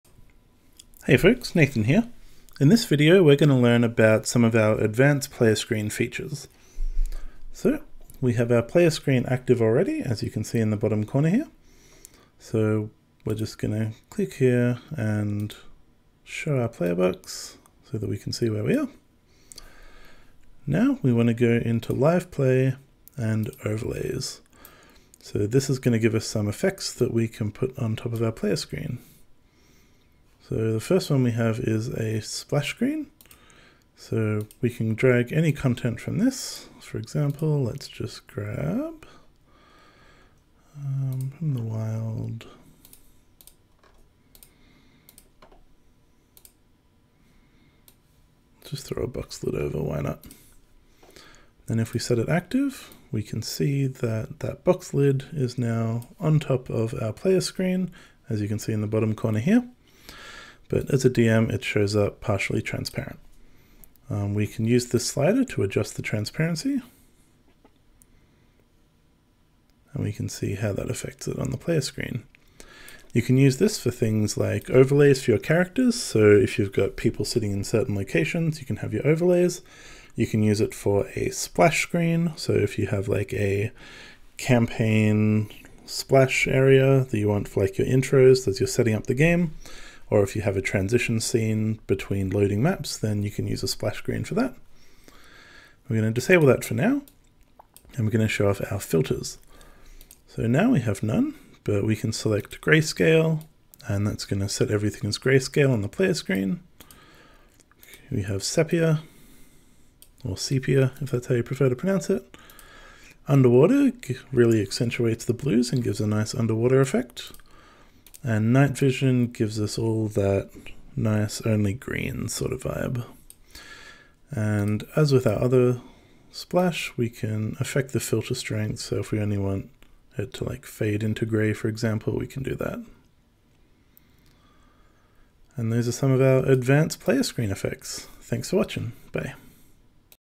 Arkenforge Tutorial - Advanced player screen functions
This tutorial shows the Player Screen Overlay and Player Screen Filter features